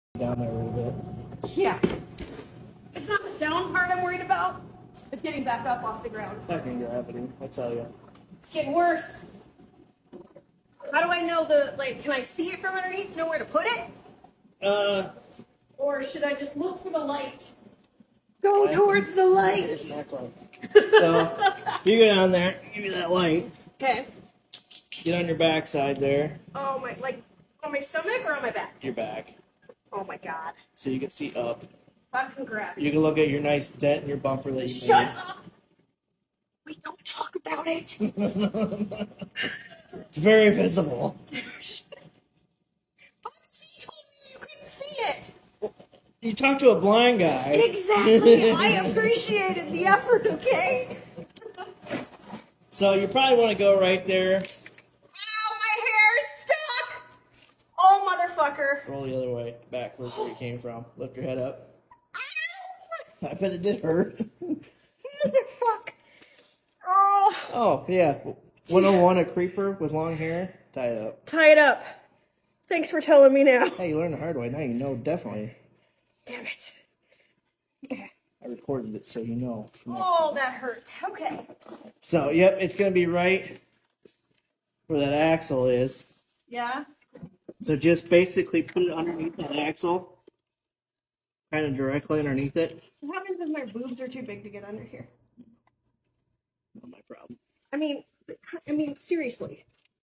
Learning to change the fuel filter in my Ram 2500
Gandalf is the name of my 2018 Ram 2500 (he is one of the loves in my life). But he is not always fun and games, especially when it comes to paying a mechanic to do basic maintenance on the road. The last time I had to change the fuel filters in Gandalf it was over $500. Thanks to my diesel mechanic brother, for my birthday he taught me how to change them myself so I could do it for just the cost of the filters.

This is glimpse into my childhood, my brother and I have always had a comical dynamic. Enjoy the fruits of my pain!